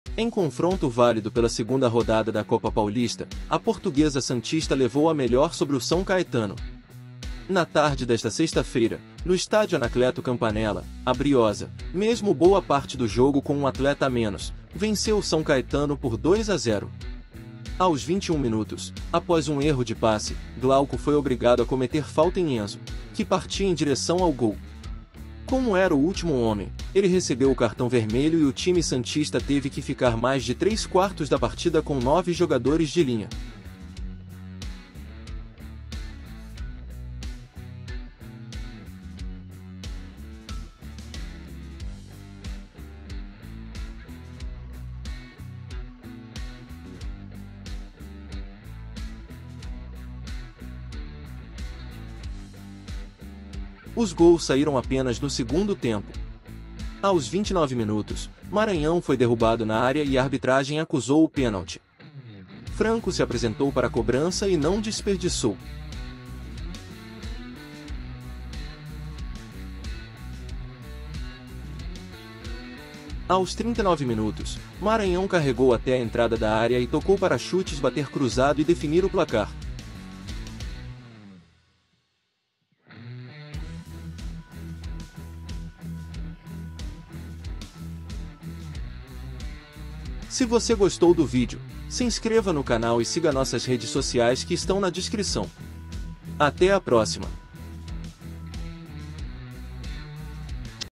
GOLS SÃO CAETANO X PORTUGUESA SANTISTA | 2ª Rodada | Copa Paulista 2023
Em confronto válido pela segunda rodada da Copa Paulista, a Portuguesa Santista levou a melhor sobre o São Caetano. Na tarde desta sexta-feira, no estádio Anacleto Campanella, a Briosa, mesmo boa parte do jogo com um atleta a menos, venceu o São Caetano por 2 a 0.